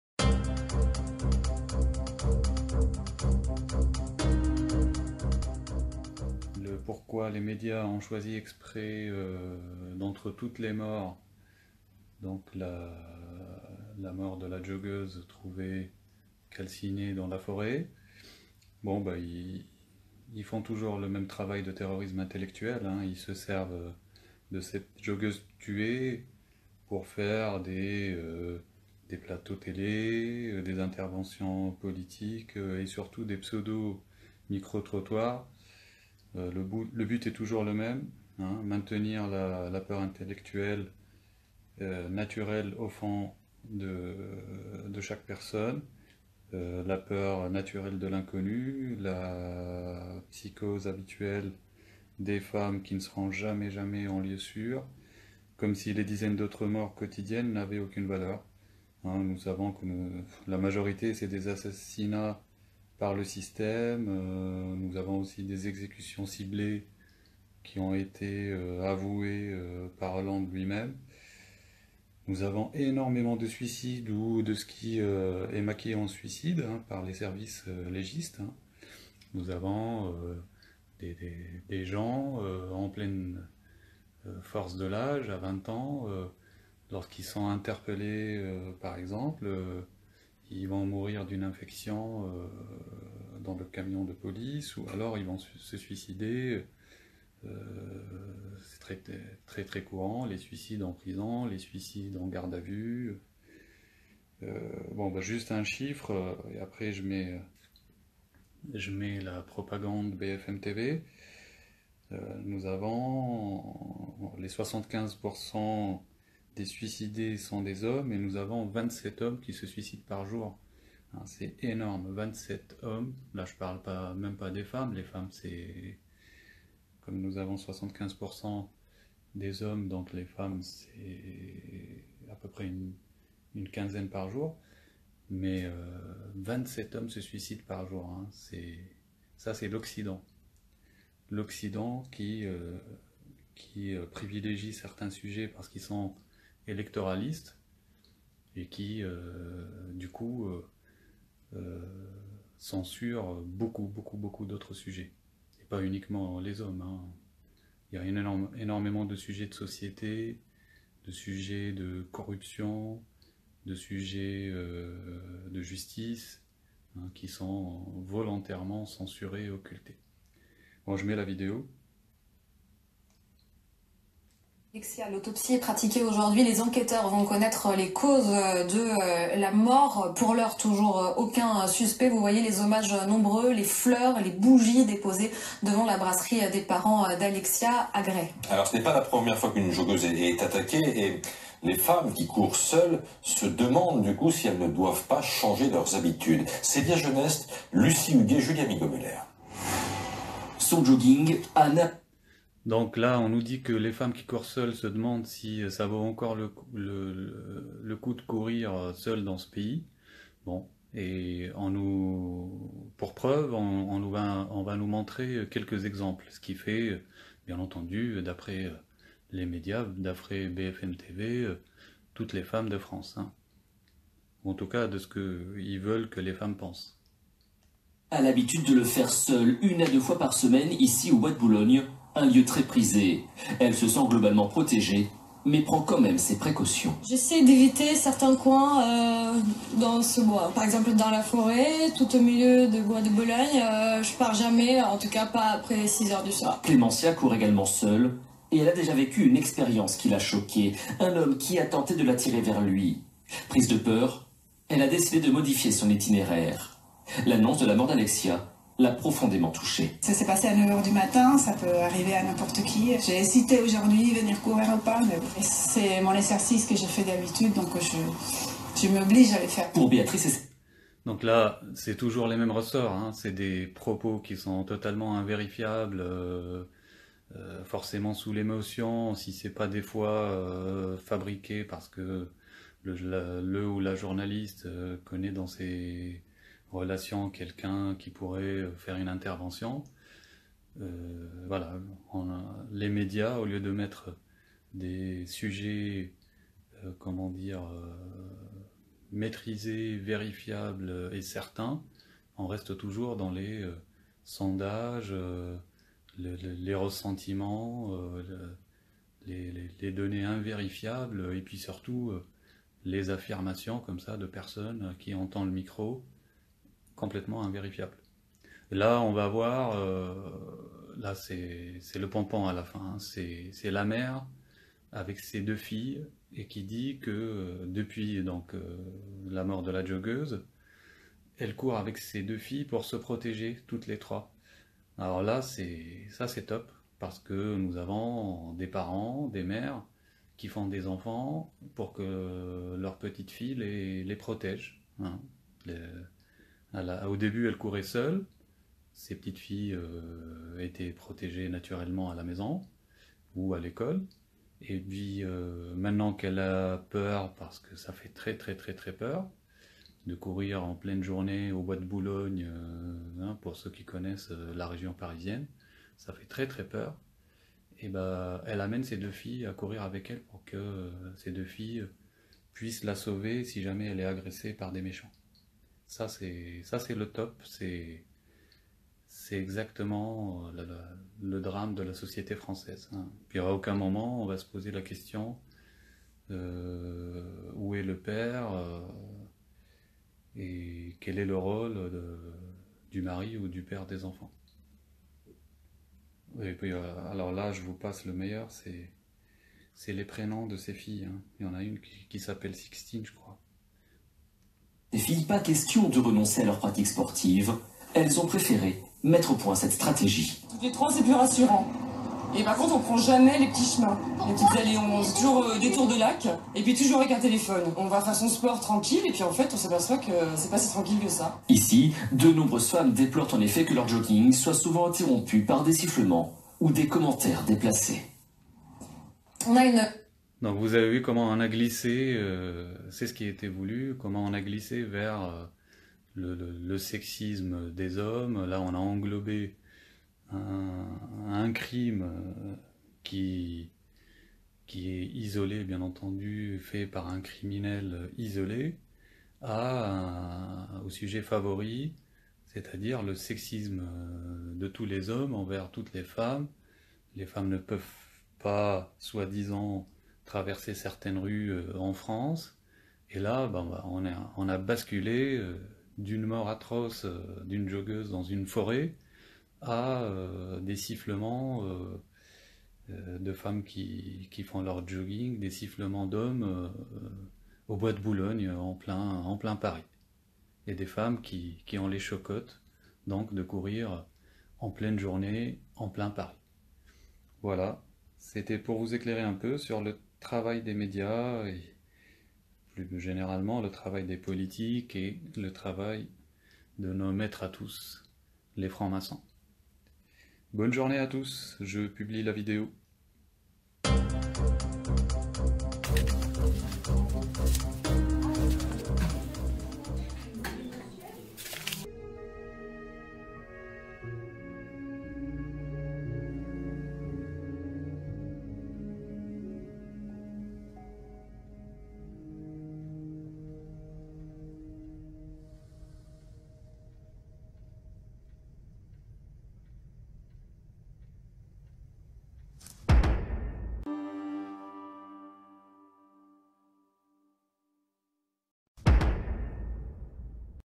CritiqueMM021117 : Manège habituel pour tirer profit de la joggeuse tuée Alexia
L'utilisation de la joggeuse Alexia DAVAL tuée (qui est AUSSI banquière) par les mérdias qui l'ont choisi d'entre toutes les morts violentes quotidiennes en France 🇫🇷.

VoteTRUMP VoteraiDoncFN (pour couper leurs projets et créer une révolution)

RipFlorianROBARDET suicidé 04/08/16

PèreDebout (encore) pour Nael et Lila
✊SIGNEZ PARTAGEZ MA PÉTITION✊